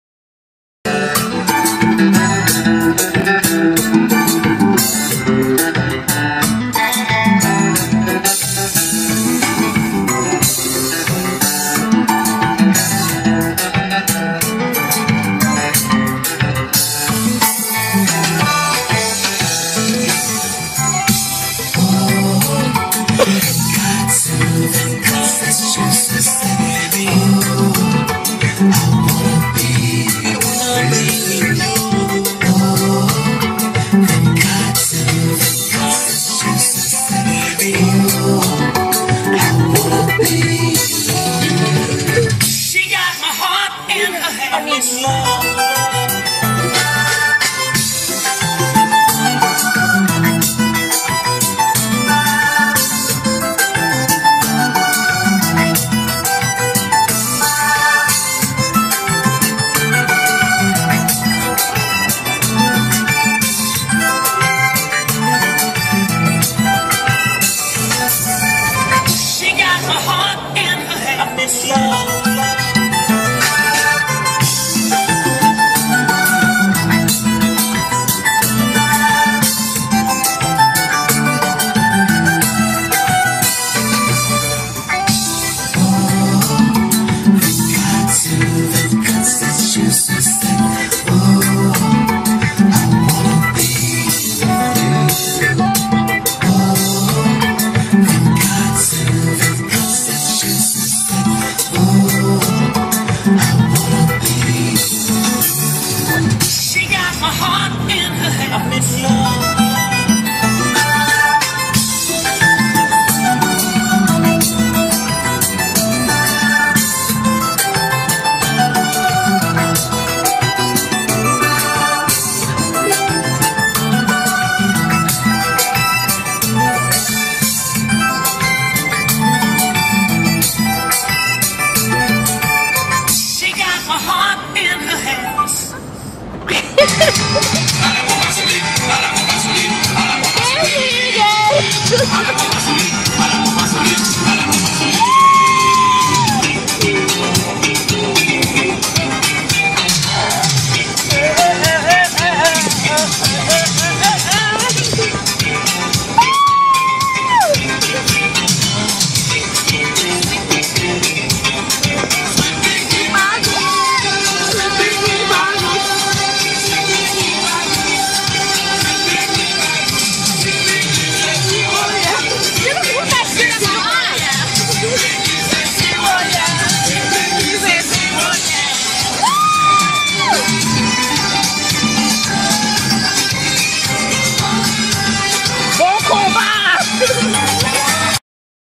Jamming To Kompa
It was a nice Spring day, and I was jamming to kompa band Ekzotic. Havinga good ol' time.